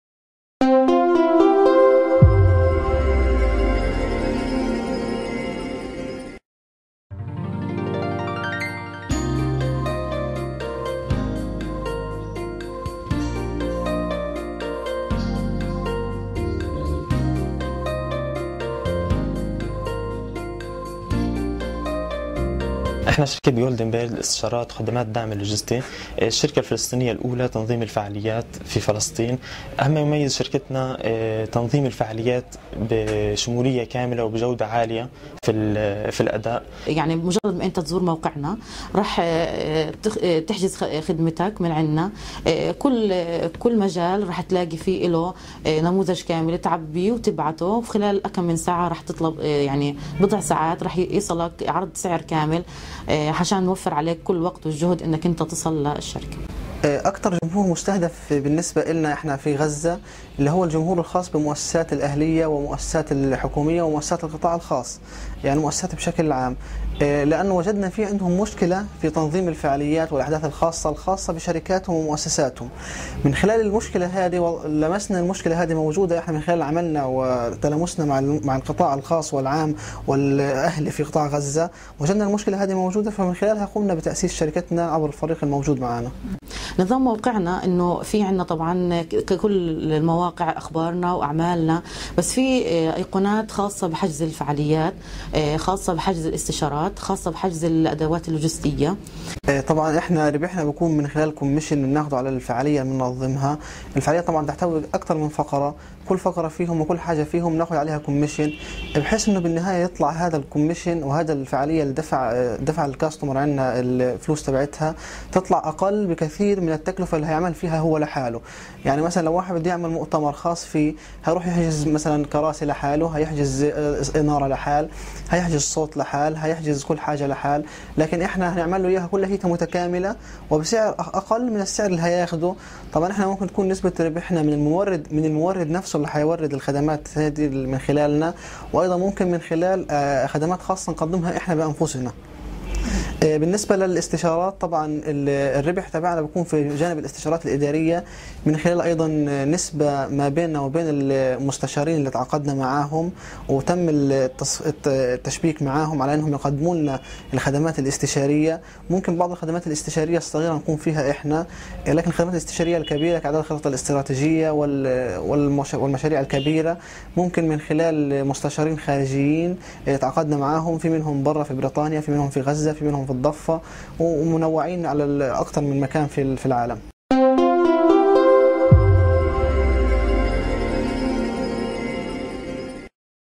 شركة فلسطينية تنظّم الفعاليات في قطاع غزة
من خلال عملهم مع القطاع والخاص والعام، لاحظ فريق "جولدين بيرد" وجود مشكلة في مجال تنظيم الفعاليات في قطاع غزة. لذلك، أسّس كلّ من محمد الهرباوي، وهبة العقاد واسحق العوضي شركة "جولدن بيرد". إقرأ المزد على ومضة.